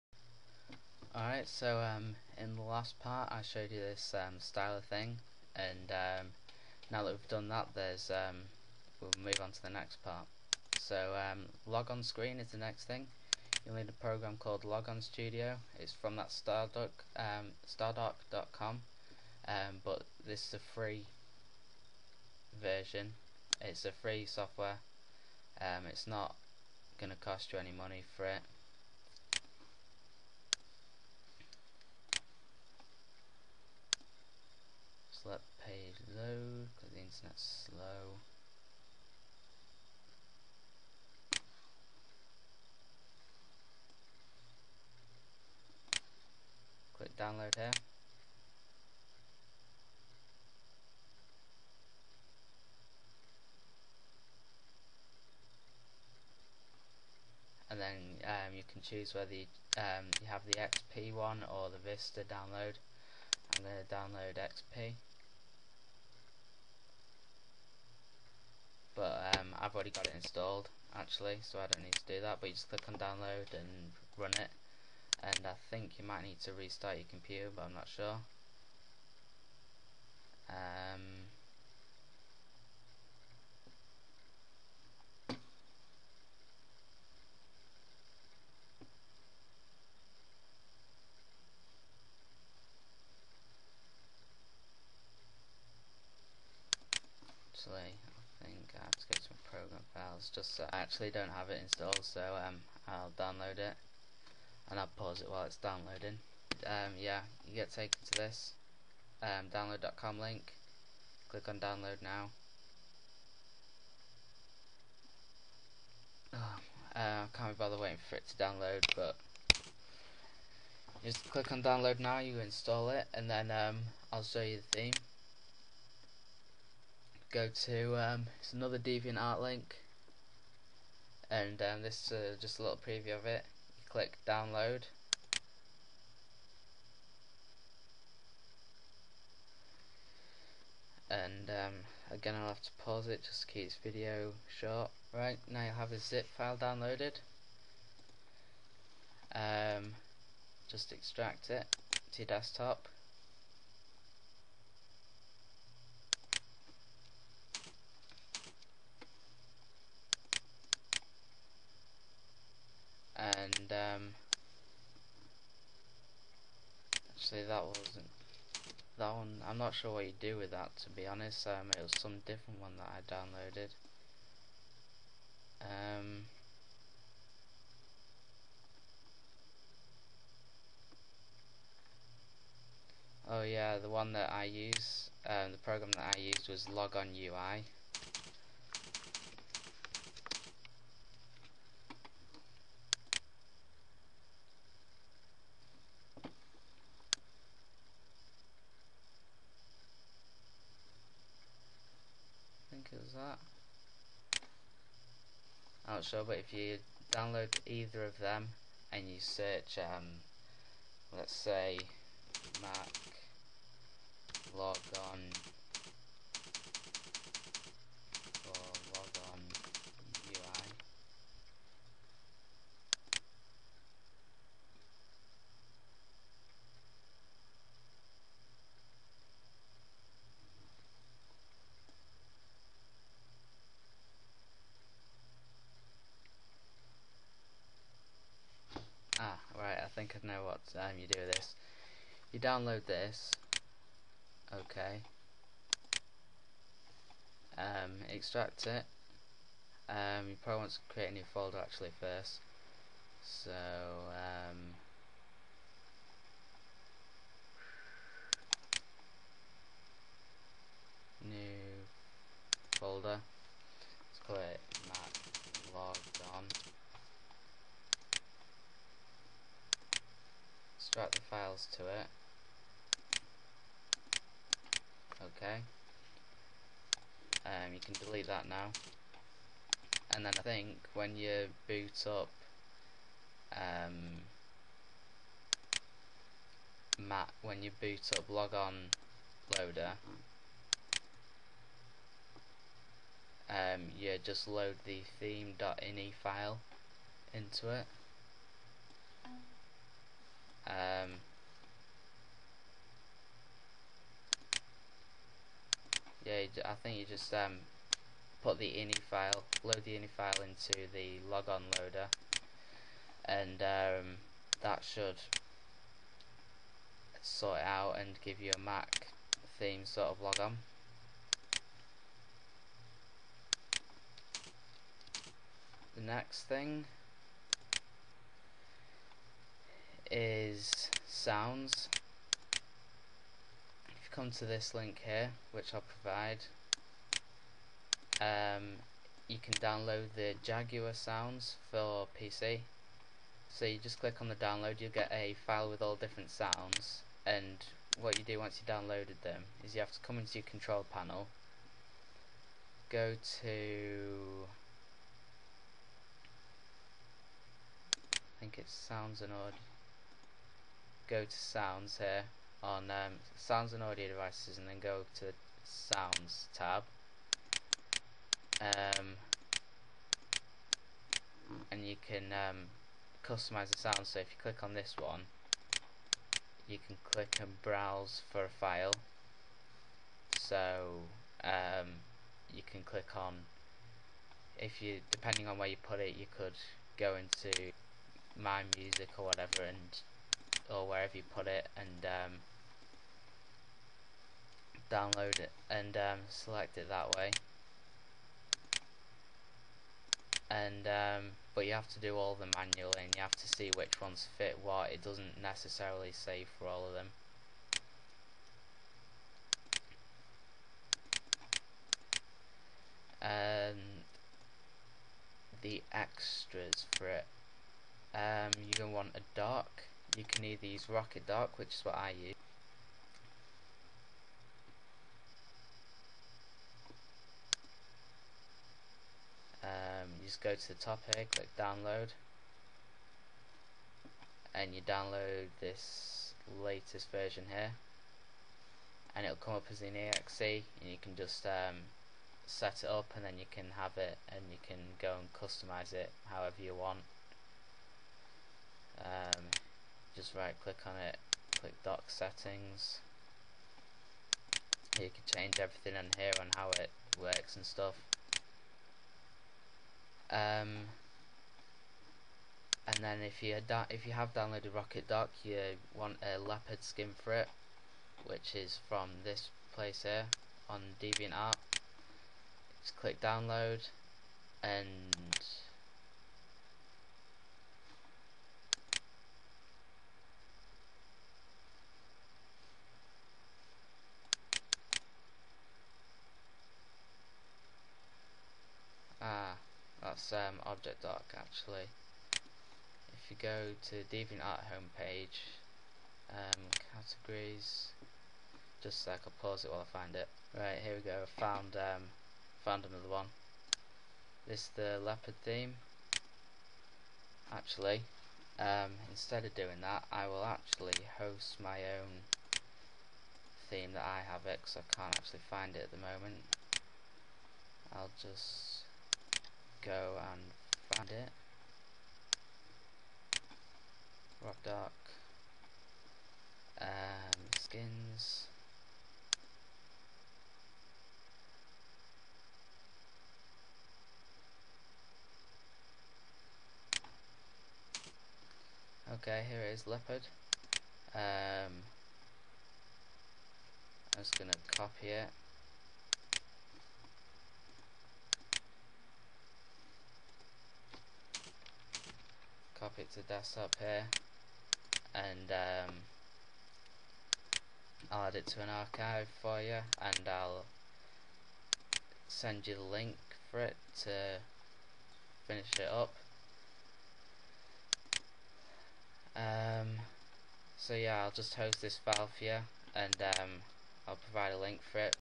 How To Turn Your Pc Into a Mac: Part 3
In this long awaited video I will show you how to transform your PC into a mac. It is split into 4 parts. The links to the other parts are posted below.

Logon Studio Skin (It is different to the one in the video but this is the one i used and its simpler to install:

Hope you enjoyed this third part of the tutorial and everything is working as it should. If you have any problems feel free to message me.

Cheers